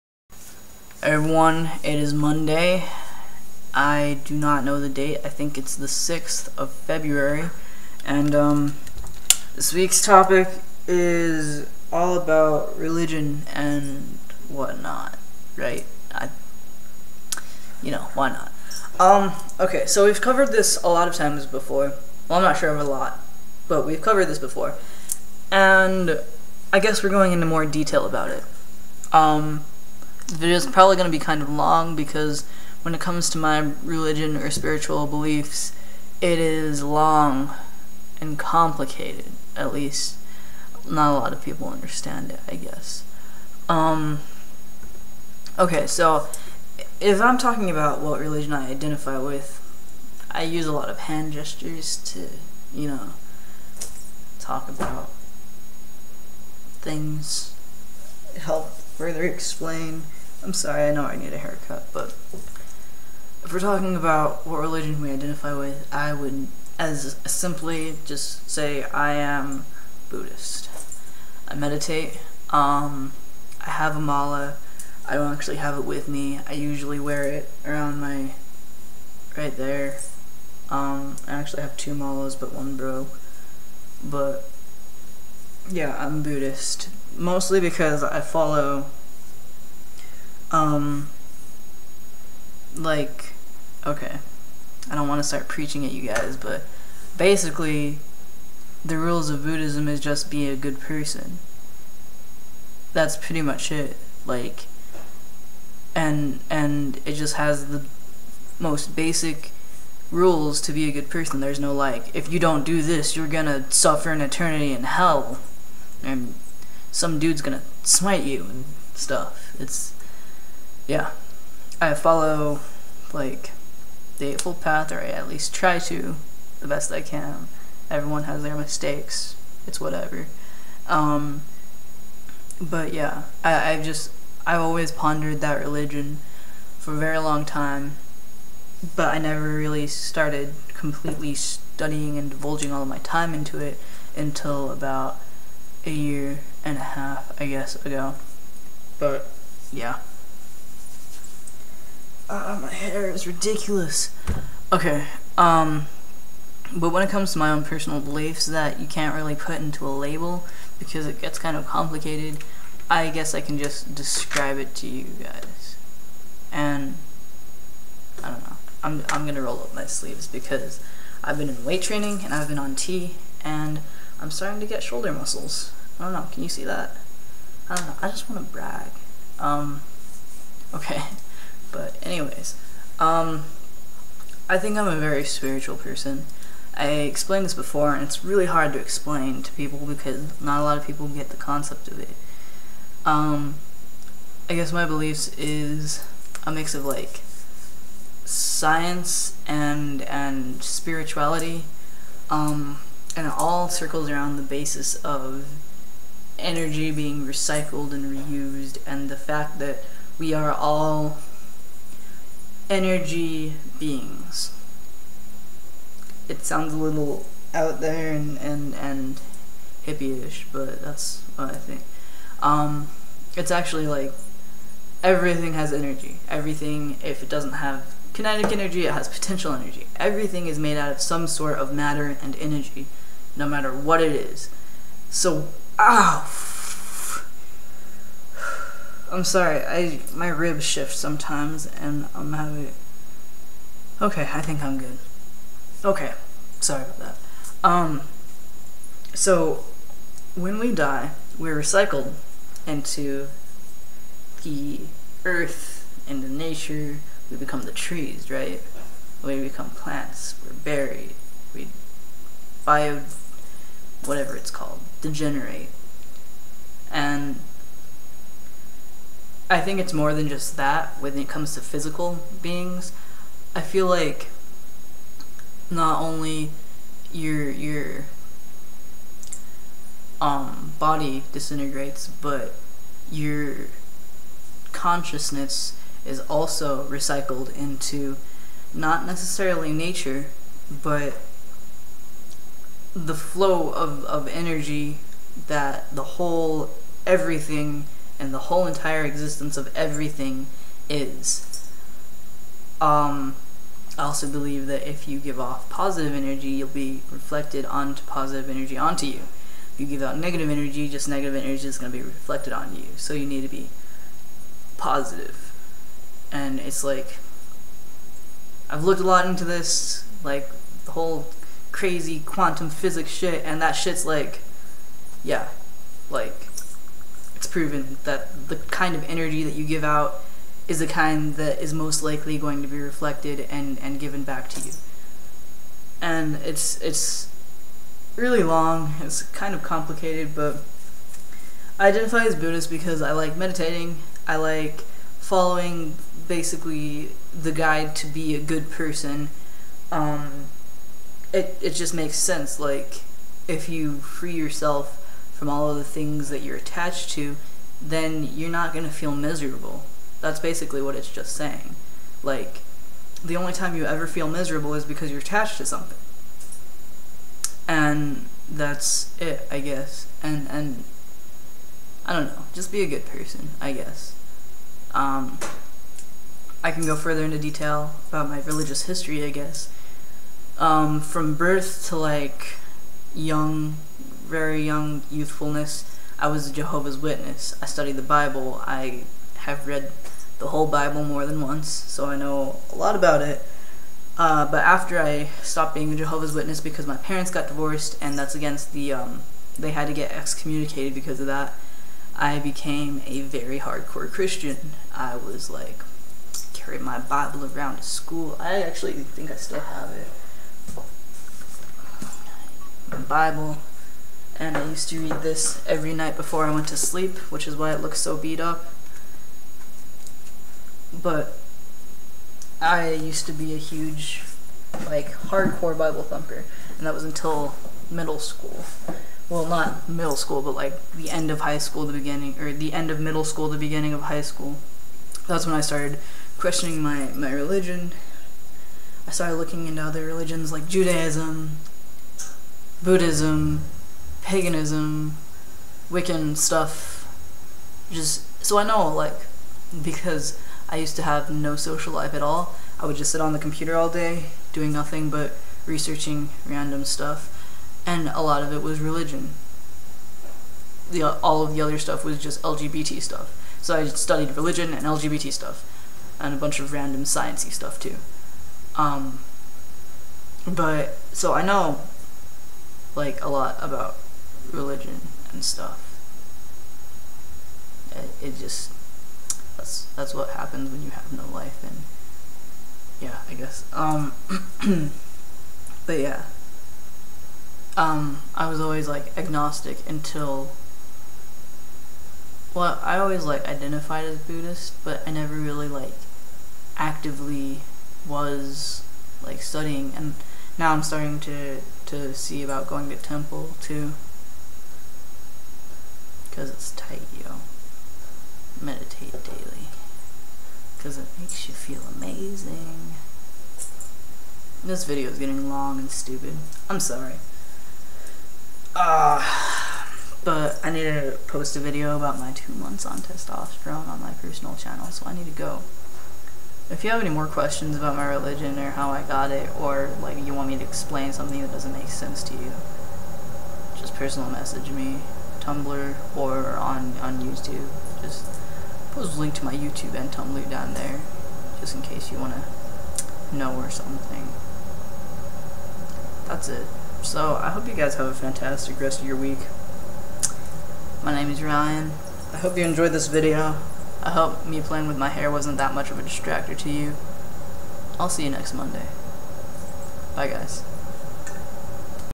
All About Religion!
This week's topic is about religion. Our religion, our spirituality, what we believe, how we discovered our religion, etc.